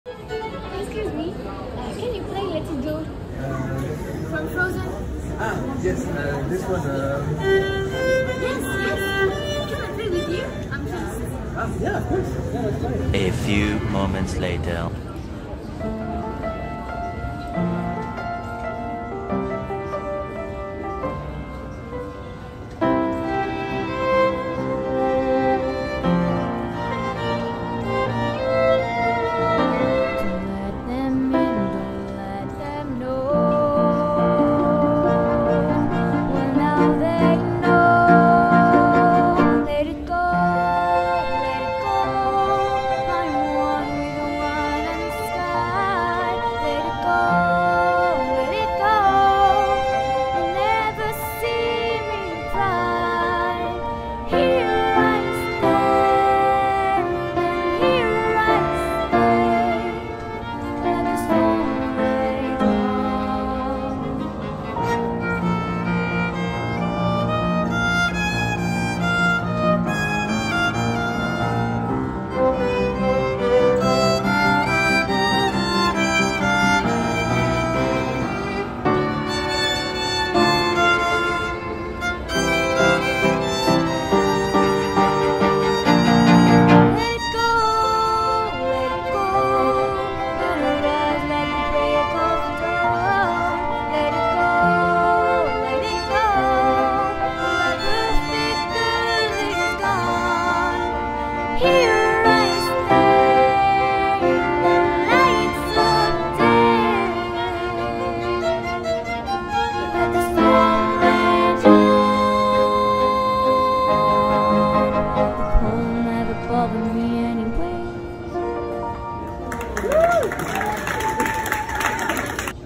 These two little girls asked to play with us, we couldn't expect how AMAZING they were..!
Musicians Guests :
🎹 Indiya Borsalerro Hermann
🎻 Augustine Borsalerro Hermann